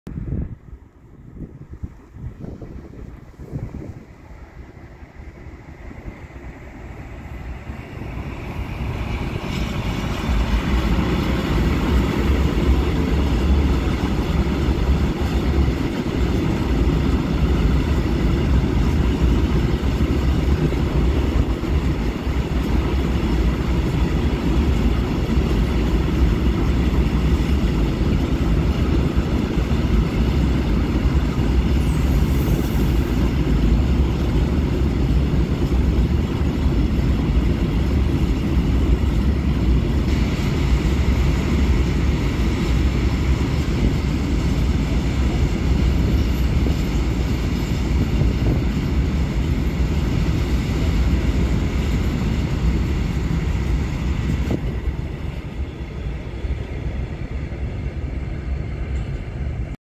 E6ACTad-026 [ZPK Szumowo]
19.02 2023 | Sokółka | Dragon2-026 w barwach spółki NATRIX wjeżdża na sokólską stację, skąd pociągnie skład na załadunek do Drahli.

Jeśli Ci się spodobało zostaw łapkę i subskrypcję.